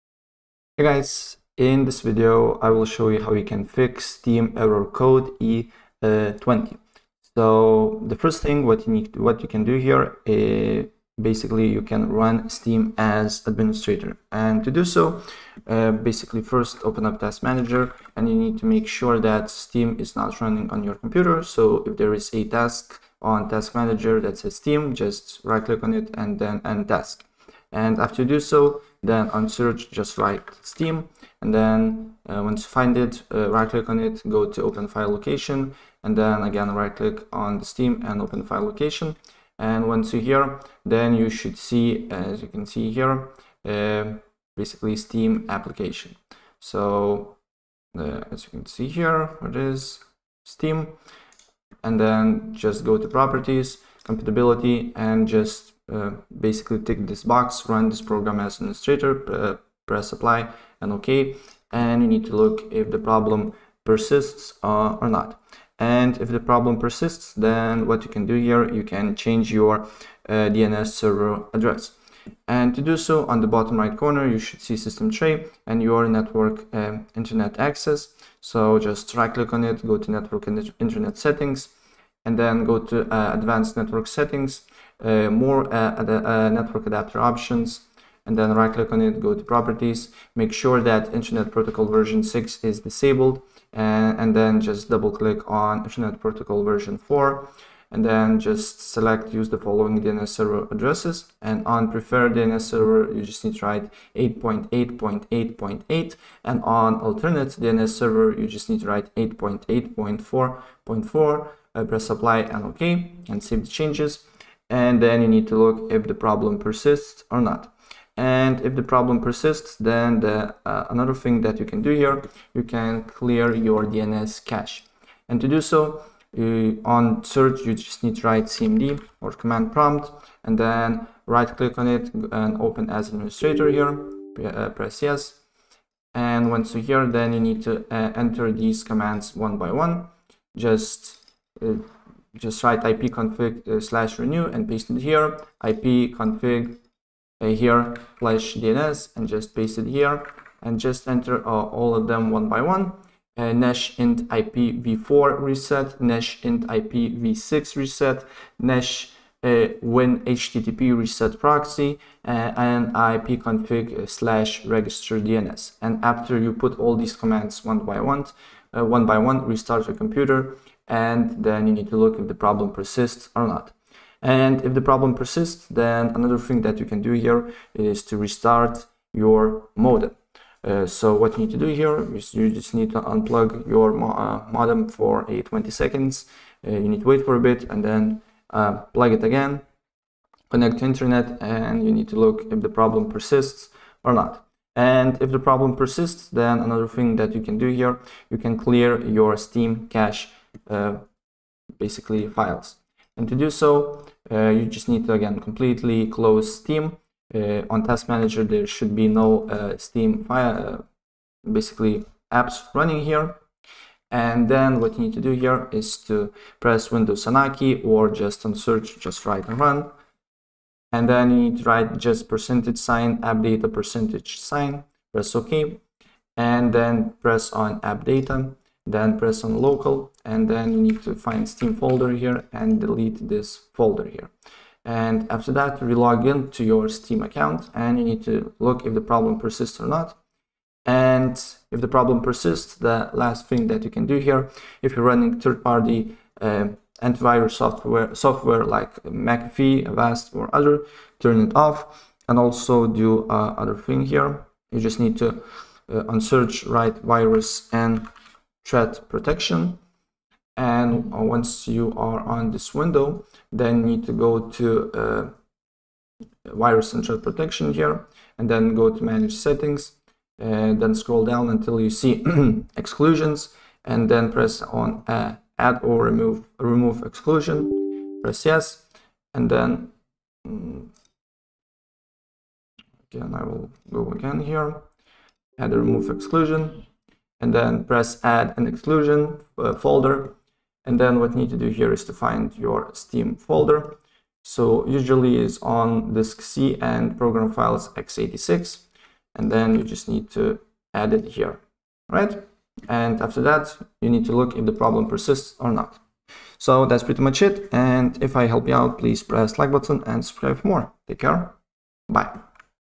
How To Fix Steam Error Code E20 - Fix Steam Connection Problem
In this video I will show you How To Fix Steam Error Code E20 - Fix Steam Connection Problem It's really easy and it will take you less than a minute to do it!